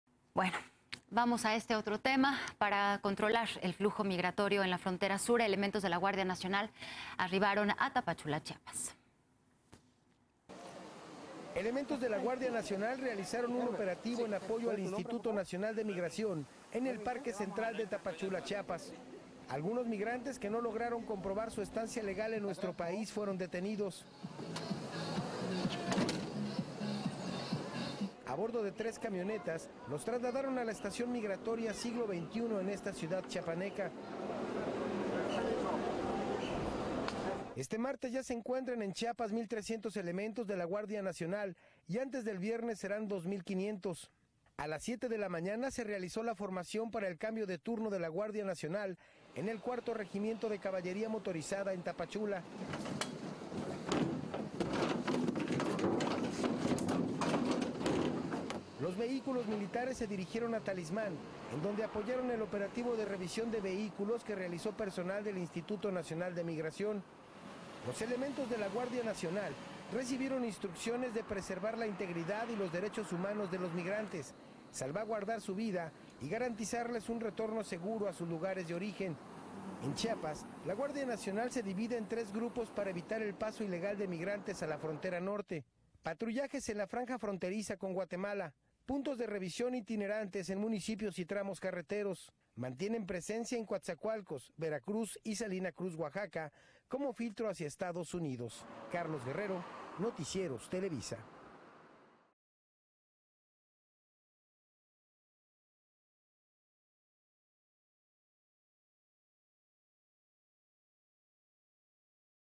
Guardia Nacional en Chiapas; así opera la Guardia Nacional contra migrantes - Las Noticias
Para controlar el flujo migratorio en la frontera sur, elementos de la Guardia Nacional arribaron a Tapachula, Chiapas.